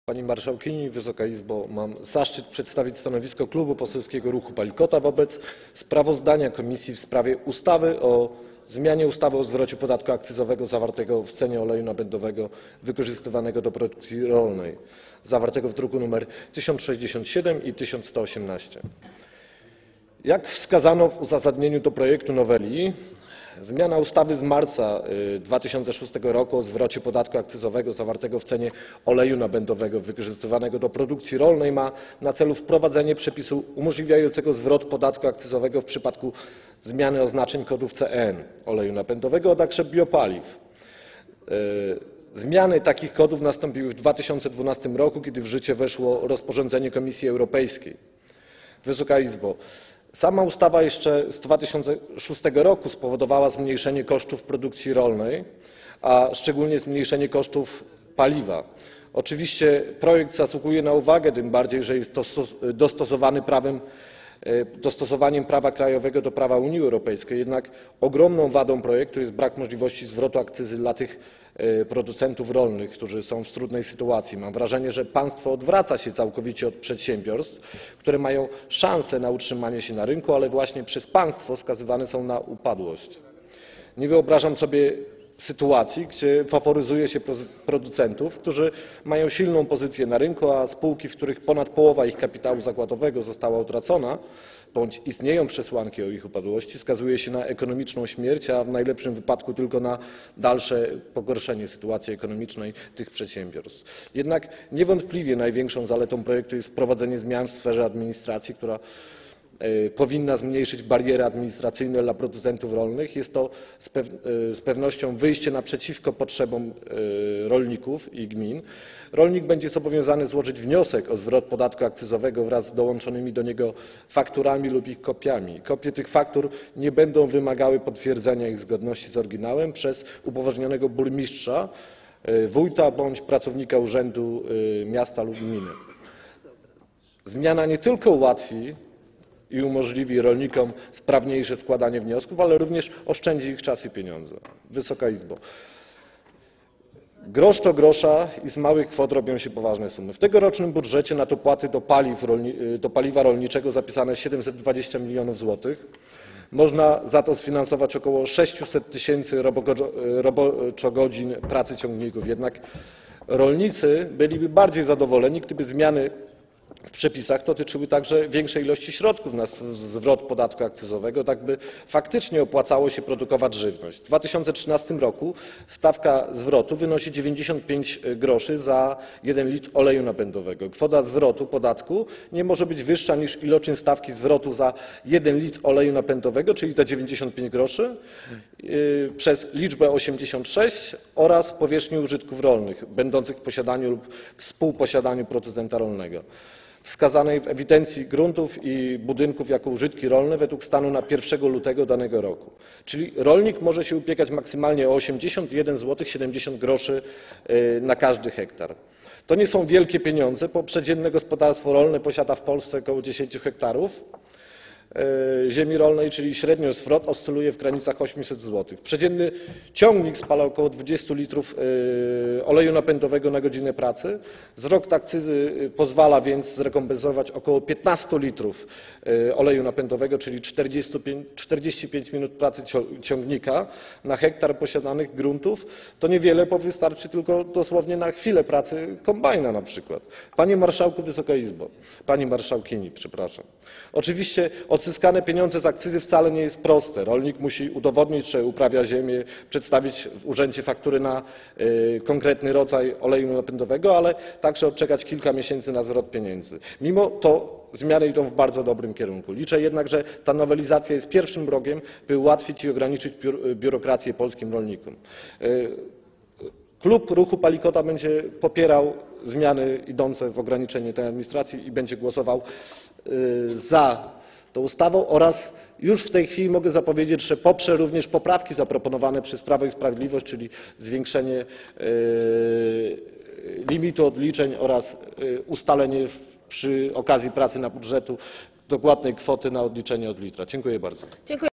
[207/318] Adam Rybakowicz: Pani Marszałkini! Wysoka Izbo! Mam zaszczyt przedstawić stanowisko K...
Posiedzenie Sejmu nr 35
6 marca 2013 r. 17:35:38
Poseł Adam Rybakowicz:

Pani Marszałkini! Wysoka Izbo! Mam zaszczyt przedstawić stanowisko Klubu Poselskiego Ruch Palikota wobec sprawozdania komisji o projekcie ustawy o zmianie ustawy o zwrocie podatku akcyzowego zawartego w cenie oleju napędowego wykorzystywanego do produkcji rolnej, druki nr 1067 i 1118.

Jak wskazano w uzasadnieniu do projektu noweli, zmiana ustawy z marca 2006 r. o zwrocie podatku akcyzowego zawartego w cenie oleju napędowego wykorzystywanego do produkcji rolnej ma na celu wprowadzenie przepisu umożliwiającego zwrot podatku akcyzowego w przypadku zmiany oznaczeń kodów CN oleju napędowego, a także biopaliw. Zmiany takich kodów nastąpiły w 2012 r., kiedy w życie weszło rozporządzenie Komisji Europejskiej.

Wysoka Izbo! Sama ustawa -- jeszcze ta z 2006 r. -- spowodowała zmniejszenie kosztów produkcji rolnej, a szczególnie zmniejszenie kosztów paliwa. Oczywiście projekt zasługuje na uwagę, tym bardziej że dostosowuje prawo krajowe do prawa Unii Europejskiej. Jednak ogromną wadą projektu jest brak możliwości zwrotu akcyzy dla tych producentów rolnych, którzy są w trudnej sytuacji. Mam wrażenie, że państwo odwraca się całkowicie od przedsiębiorstw, które mają szansę na utrzymanie się na rynku, ale właśnie przez państwo skazywane są na upadłość. Nie wyobrażam sobie sytuacji faworyzowania producentów, którzy mają silną pozycję na rynku, a skazywania spółek, w których ponad połowa kapitału zakładowego została utracona, bądź istnieją przesłanki o ich upadłości, na ekonomiczną śmierć, a w najlepszym wypadku na dalsze pogorszenie ich sytuacji ekonomicznej.

Jednak niewątpliwie największą zaletą projektu jest wprowadzenie zmian w sferze administracji. Powinno to zmniejszyć barierę administracyjną dla producentów rolnych. Jest to z pewnością wyjście naprzeciw potrzebom rolników i gmin. Rolnik będzie zobowiązany złożyć wniosek o zwrot podatku akcyzowego wraz z dołączonymi do niego fakturami lub ich kopiami. Kopie tych faktur nie będą wymagały potwierdzenia ich zgodności z oryginałem przez upoważnionego burmistrza, wójta bądź pracownika urzędu miasta lub gminy. Zmiana ta nie tylko ułatwi i umożliwi rolnikom sprawniejsze składanie wniosków, ale również oszczędzi ich czas i pieniądze.

Wysoka Izbo! Grosz do grosza i z małych kwot robią się poważne sumy. W tegorocznym budżecie na dopłaty do paliwa rolniczego zapisane jest 720 mln zł, można za to sfinansować około 600 tys. roboczogodzin pracy ciągników, jednak rolnicy byliby bardziej zadowoleni, gdyby zmiany w przepisach dotyczyły także większej ilości środków na zwrot podatku akcyzowego, tak by faktycznie opłacało się produkować żywność. W 2013 r. stawka zwrotu wynosi 95 gr za 1 litr oleju napędowego. Kwota zwrotu podatku nie może być wyższa niż iloczyn stawki zwrotu za 1 litr oleju napędowego, czyli 95 gr, przez liczbę 86 oraz powierzchnię użytków rolnych będących w posiadaniu lub współposiadaniu producenta rolnego wskazaną w ewidencji gruntów i budynków jako użytki rolne według stanu na dzień 1 lutego danego roku. Czyli rolnik może się ubiegać maksymalnie o 81,70 zł na każdy hektar. To nie są wielkie pieniądze. W Polsce przeciętne gospodarstwo rolne posiada około 10 ha ziemi rolnej, czyli średnio zwrot oscyluje w granicach 800 zł. Przeciętny ciągnik spala około 20 litrów oleju napędowego na godzinę pracy. Zwrot akcyzy pozwala więc zrekompensować około 15 litrów oleju napędowego, czyli 45 minut pracy ciągnika, na hektar posiadanych gruntów to niewiele, bo wystarczy dosłownie tylko na chwilę pracy na przykład kombajnu.

Panie Marszałku! Wysoka Izbo! Pani Marszałkini! Przepraszam. Oczywiście odzyskanie pieniędzy z akcyzy wcale nie jest proste, rolnik musi udowodnić, że uprawia ziemię, przedstawić w urzędzie faktury na konkretny rodzaj oleju napędowego, ale także odczekać kilka miesięcy na zwrot pieniędzy, ...